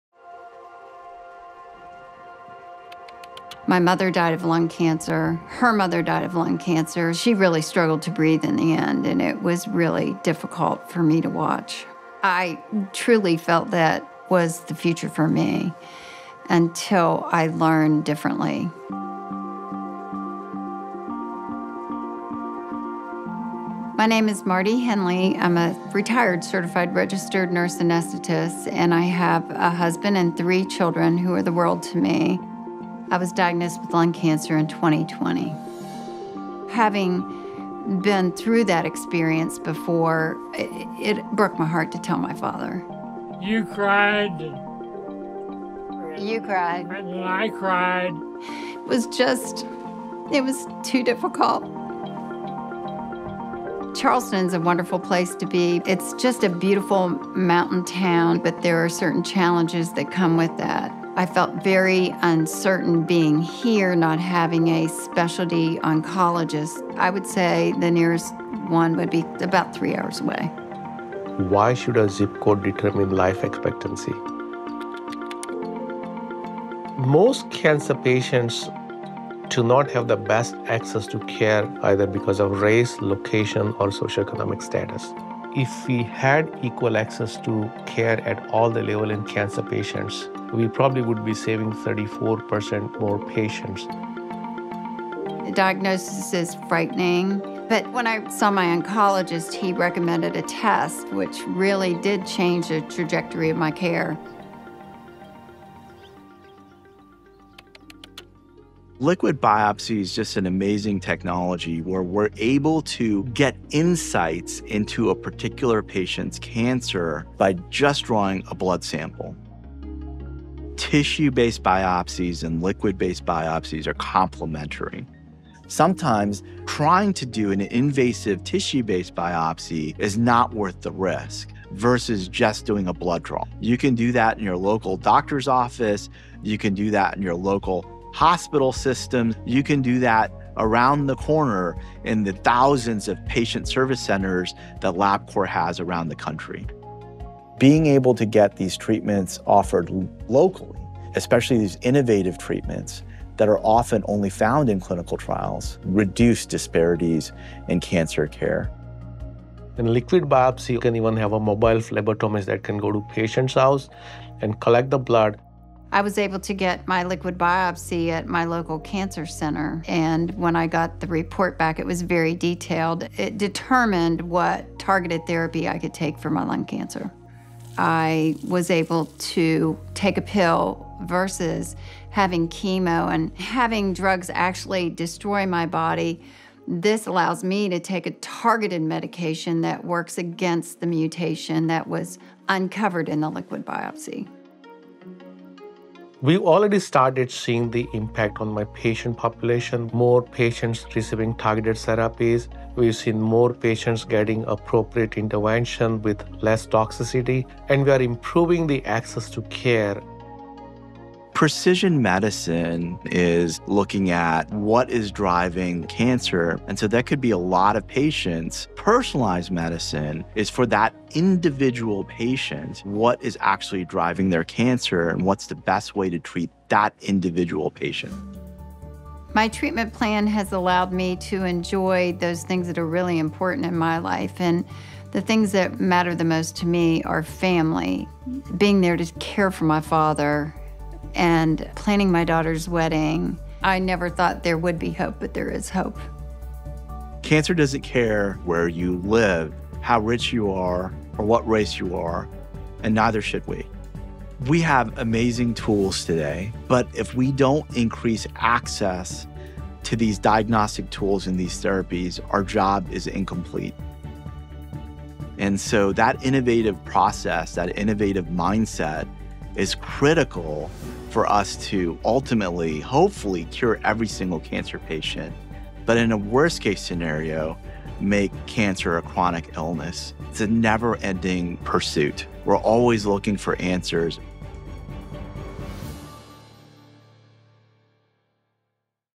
How a blood test is making cancer care more equitable
Cancer doesn’t care who you are or where you live, yet survival rates are often tied to a person’s access to adequate care. We want to change that. In "Zip Code & Genetic Code: Improving Access to Breakthrough Cancer Care," we partnered with WIRED to share how our advancements in science and technology are bringing personalized cancer care within reach for more people.